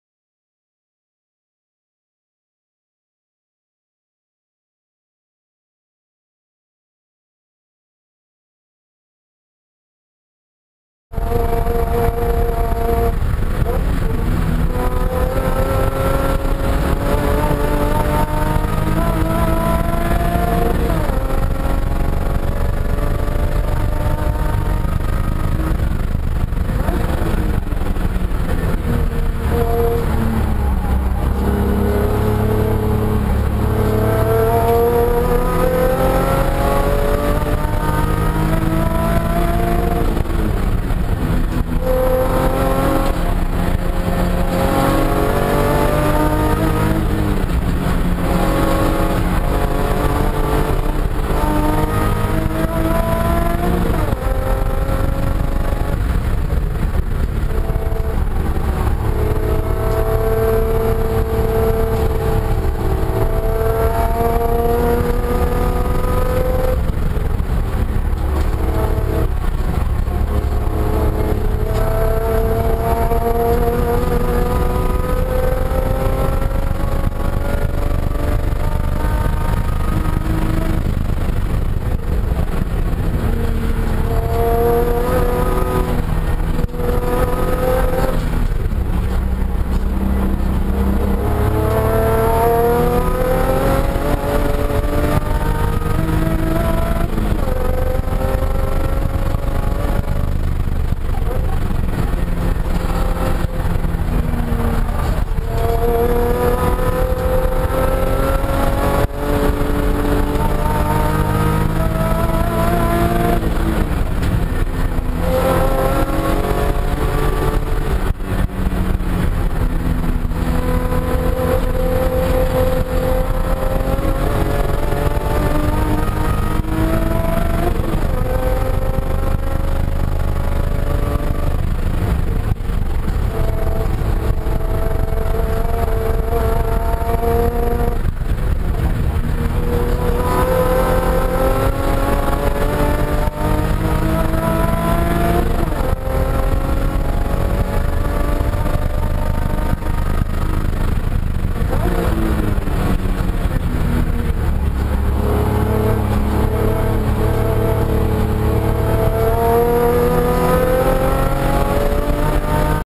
Hotlaps with Southpaw at Chuckwalla - E36 M3 HGMS/BMW of El Cajon
One of my quicker laps of the day. First time at the track with the new Tig welded prepared class cage by HG Design and HG Motorsports.

The M3 dynoed 212whp on a DynoDynamics and weighs probably around 2900lbs, I've never weighed it though.